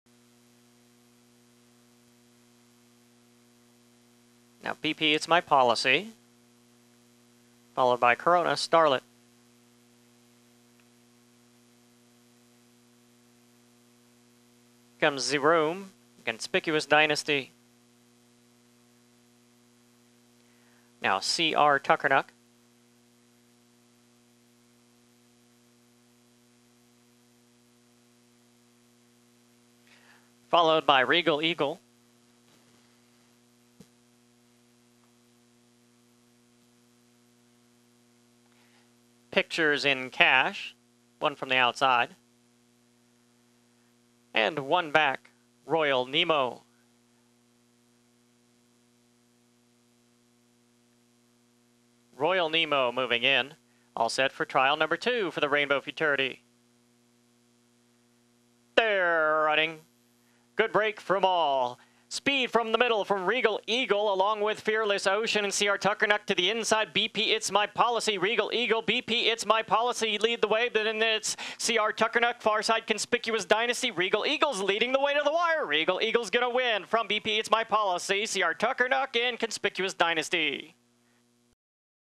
Ruidoso Futurity G1 Trial #2 July 4, 2013
Ruidoso Futurity G1 Trial #2 - Fastest Qualifier Day 1 - 400 yards :19.242
1. Reagal Eagle - Faster Qualifier
2. Bp Its My Policy - Qualifier
3. Cr Tuckernuck